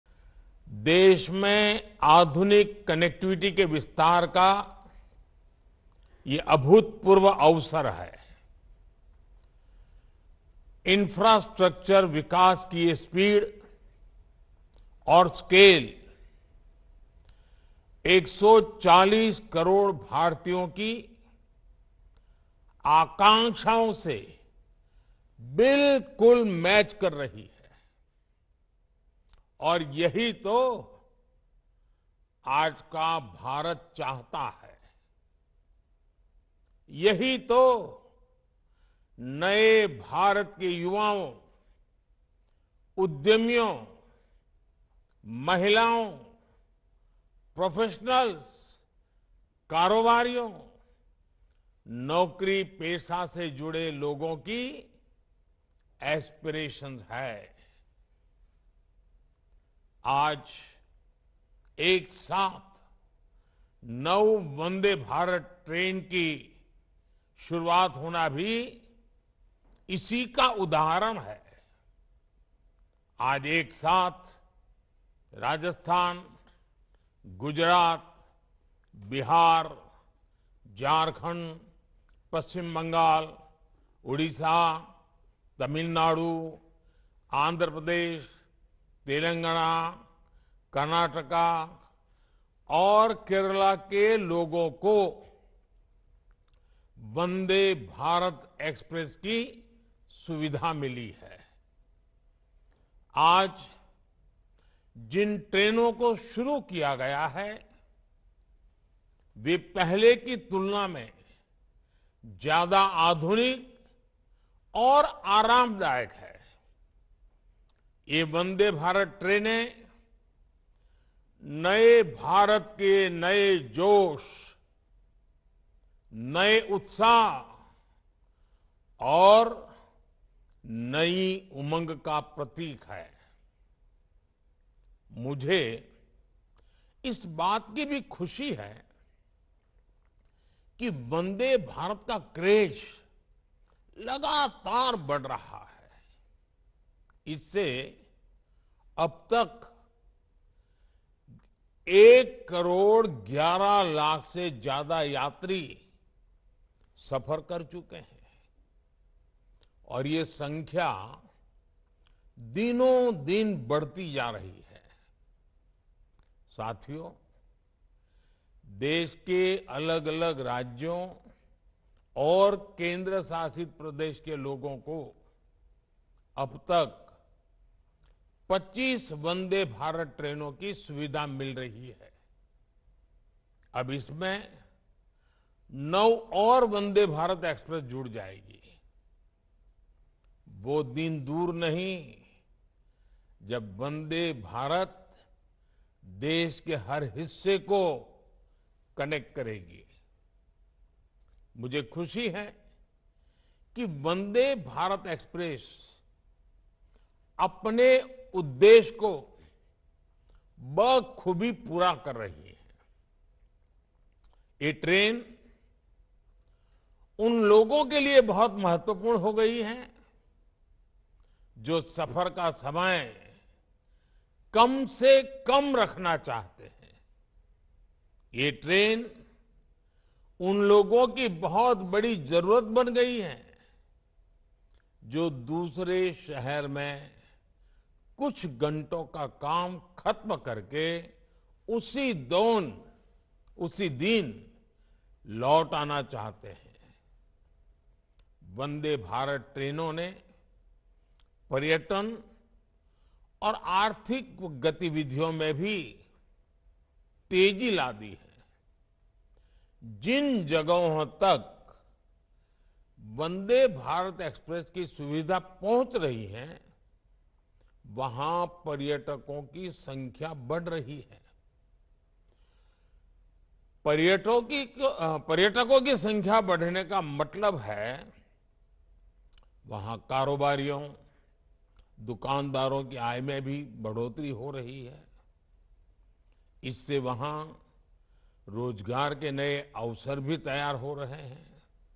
वंदे भारत ट्रेनें नए भारत के नए जोश, नए उत्साह और नई उमंग का प्रतीक: पीएम नरेन्द्र मोदी
Prime Minister Narendra Modi flagged off 9 Vande Bharat Trains across 11 states. He said that India has attained an unprecedented level of infrastructural transformation that has reimagined India' infrastructural perspective at the global level.

He added that speed and scale of infrastructure development in the country is exactly matching the aspirations of 140 crore Indians”. Prime Minister Narendra Modi said that the trains started today are more modern and comfortable. He said these Vande Bharat Trains are symbols of a new enthusiasm of new India. He expressed delight about the growing craze of Vande Bharat as he informed that more than one crore eleven lakh passengers have travelled in Vande Bharat train.